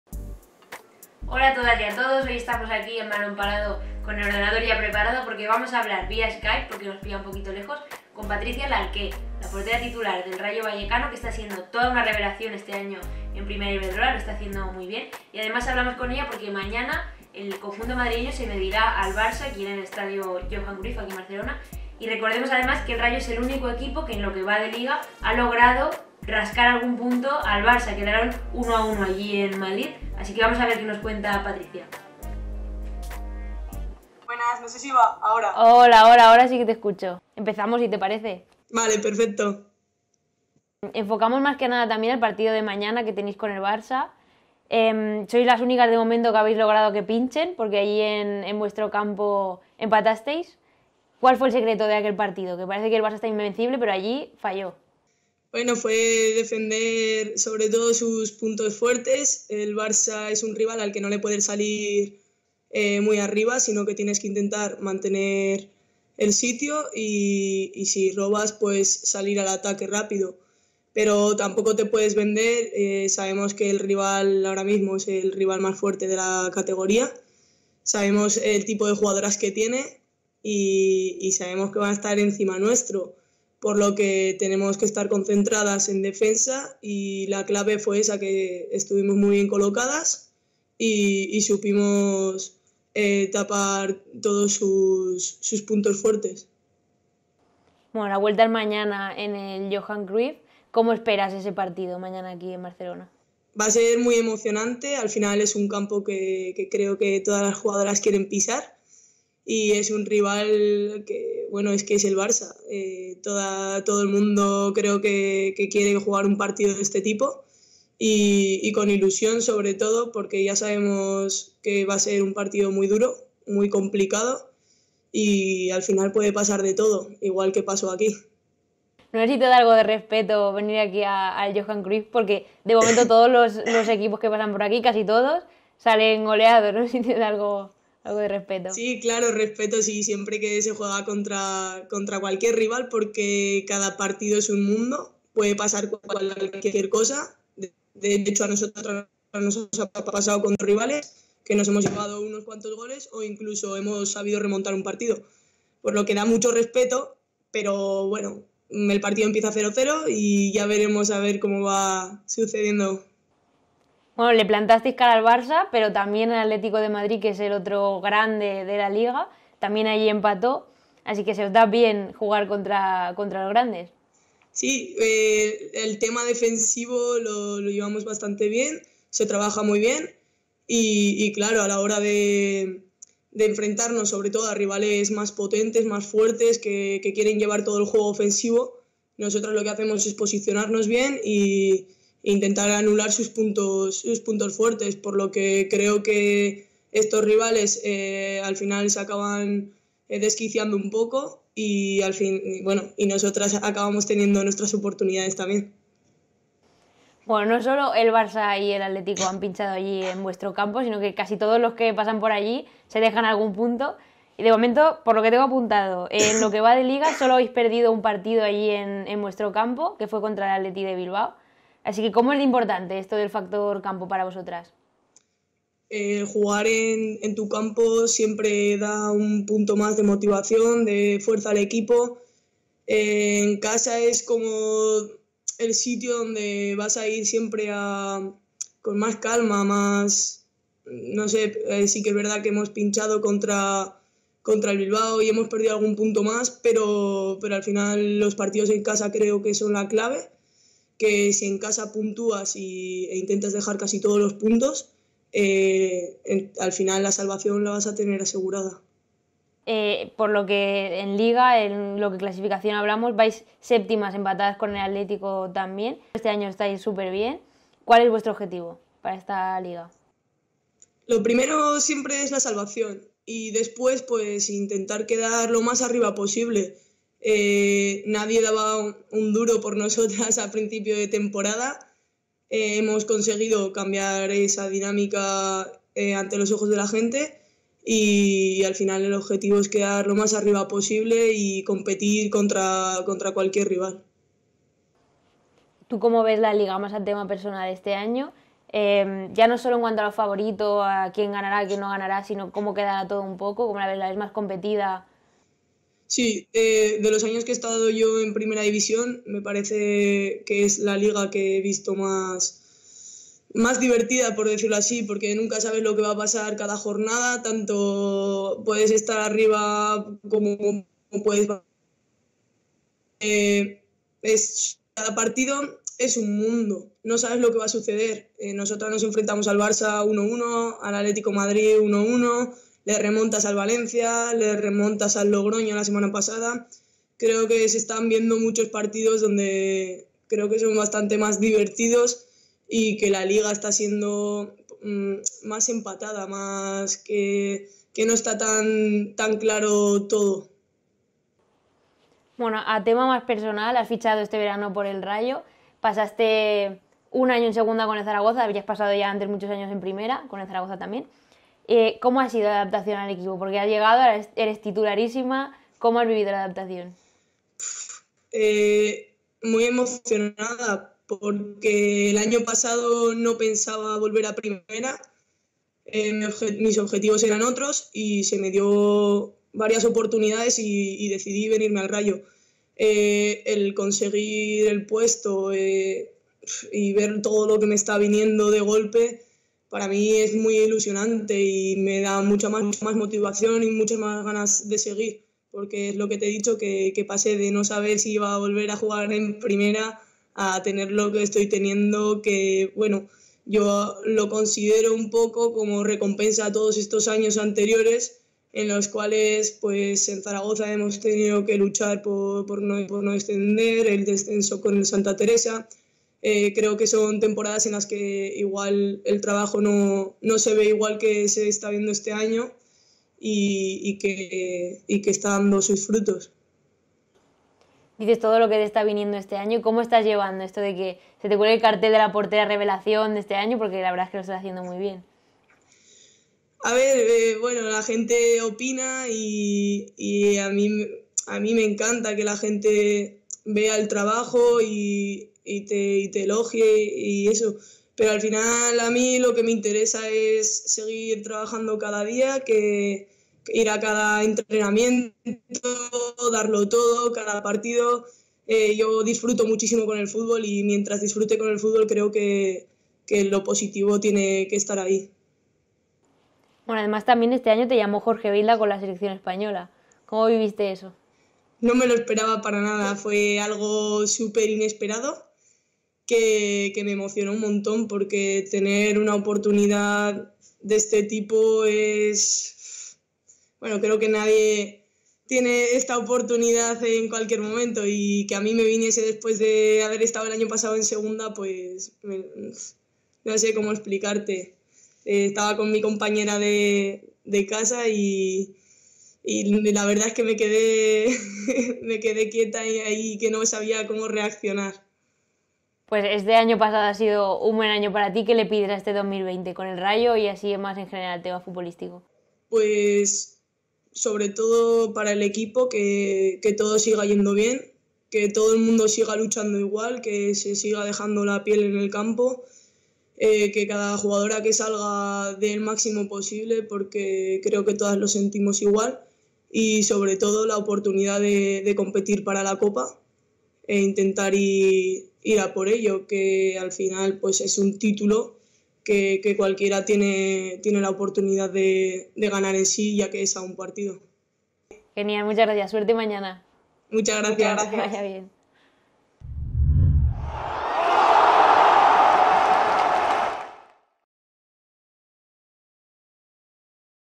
PATRICIA LARQUÉ, la portera que le plantó CARA al BARÇA
Hablamos con Patricia Larqué, portera del Rayo Vallecano, único equipo con el que el Barça ha 'pinchado' esta temporada liguera